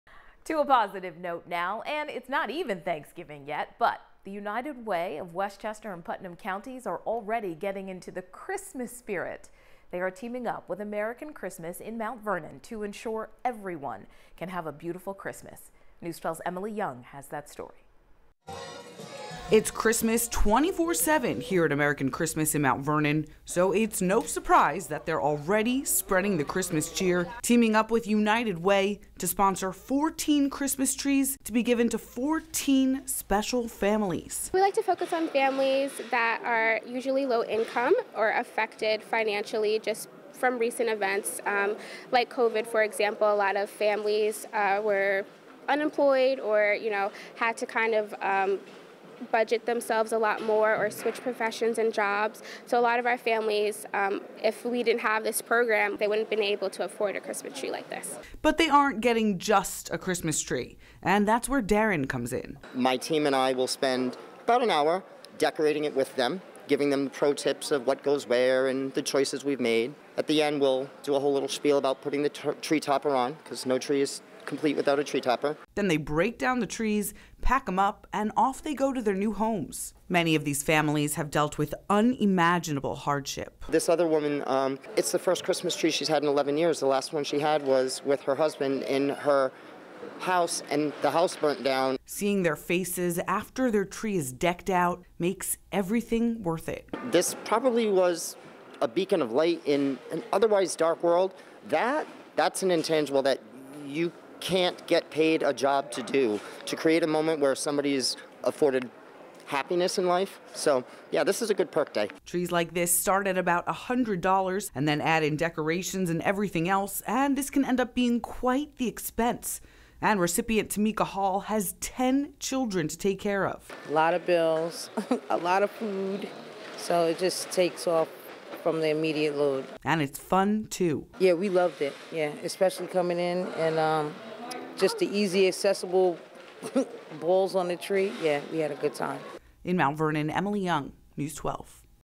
United Way, American Christmas spread holiday cheer by gifting trees to those in need
United Way of Westchester and Putnam got into the Christmas spirit by teaming up with American Christmas in Mount Vernon to ensure everyone can have a beautiful holiday.